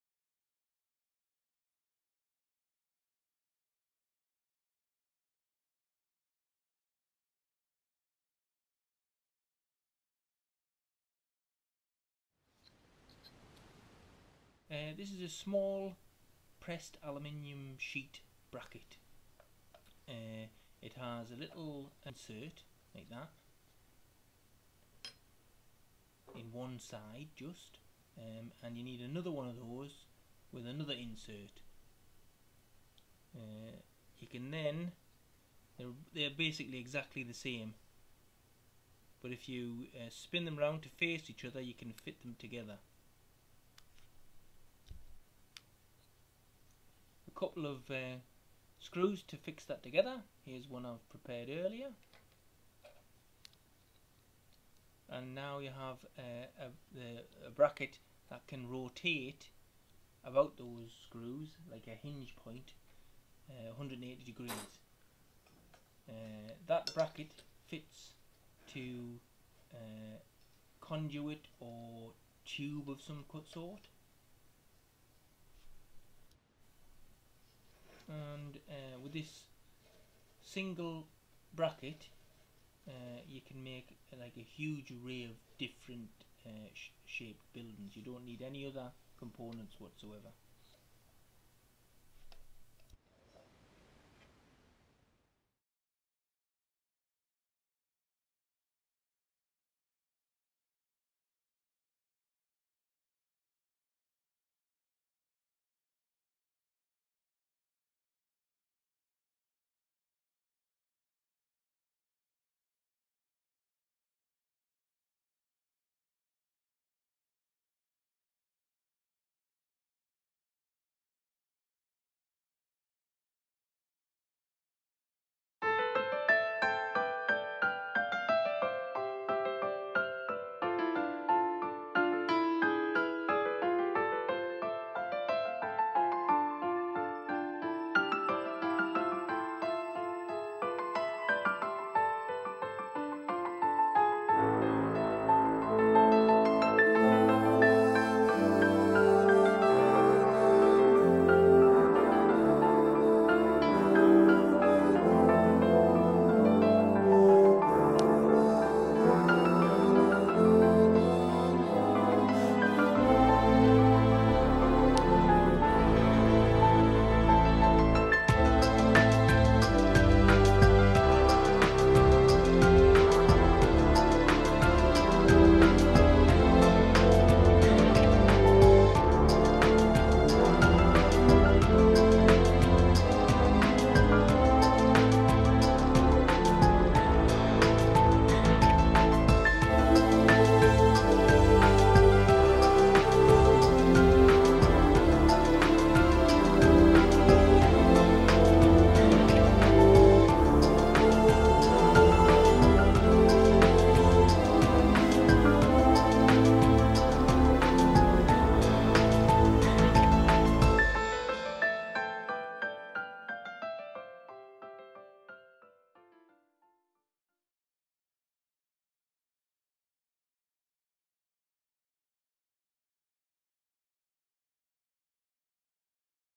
UTA brackets
This is a hub design I did for making structures using only one hub component, It's possible to make just about any shape you want not to mention a lot of other things. I will add a link to the files so you can download the sketchup files and maybe get a few 3d printed. I had about 200 made from sheet aluminum for a lighting rig project.